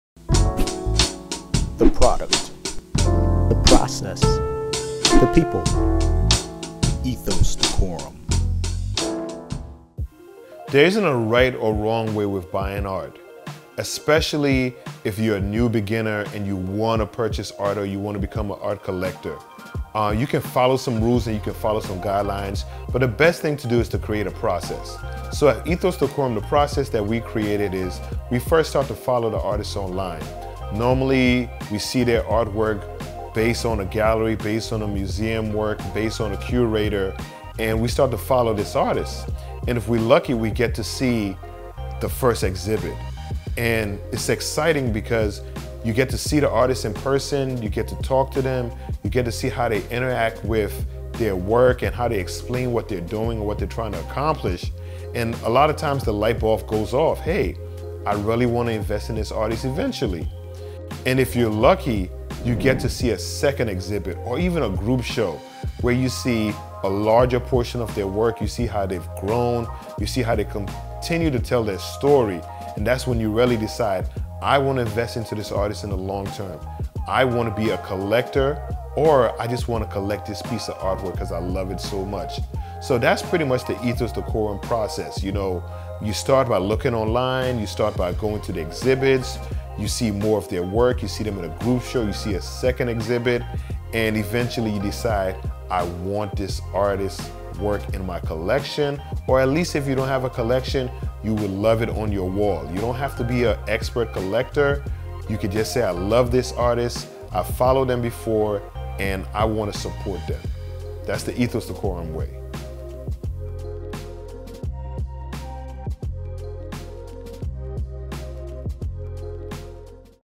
How to Buy Art
Thinking of buying art? For the average person buying art can be intimidating. There is no right or wrong way to buy art so we decided to document a process and a few things to take into consideration. The truth is just buy what you like and support the artists you like.

ETHOS DECORUM is an exclusive  content provider of fashion, art, technology and all things within today's popular culture. Our motto is the product, the process and the people